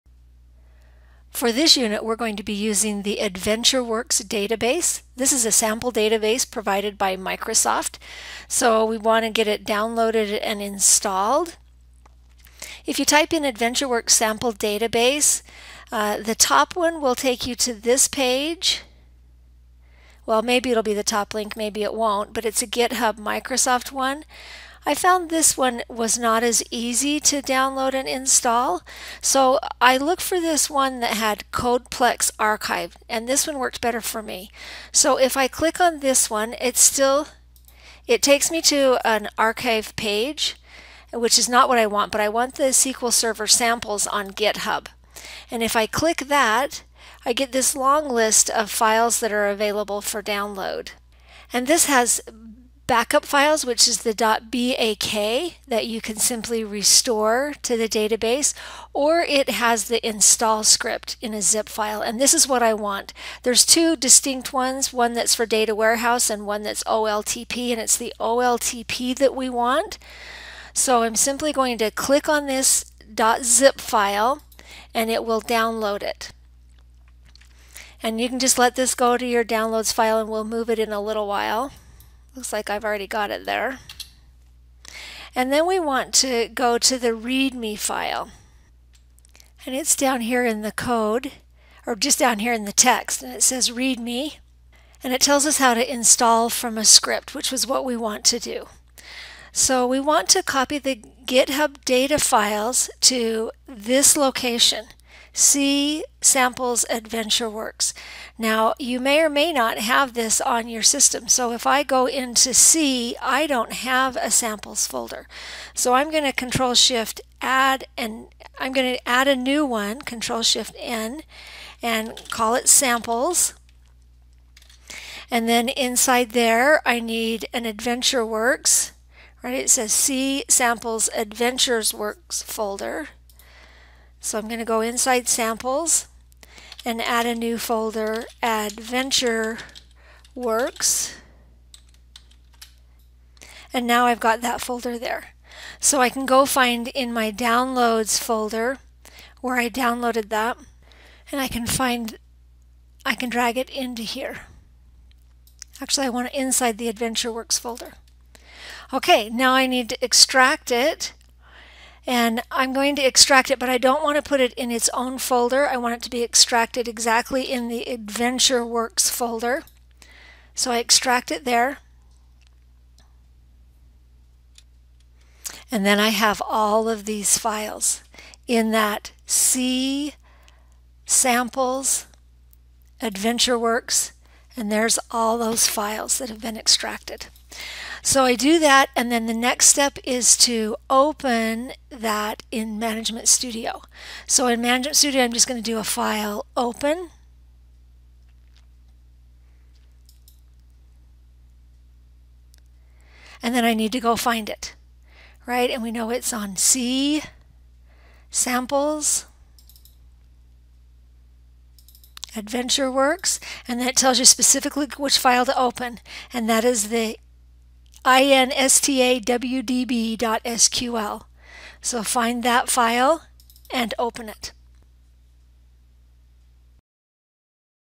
AdventureWorks, Part 1
A demonstration of how to download the script files for the SQL Server Sample Database, AdventureWorks.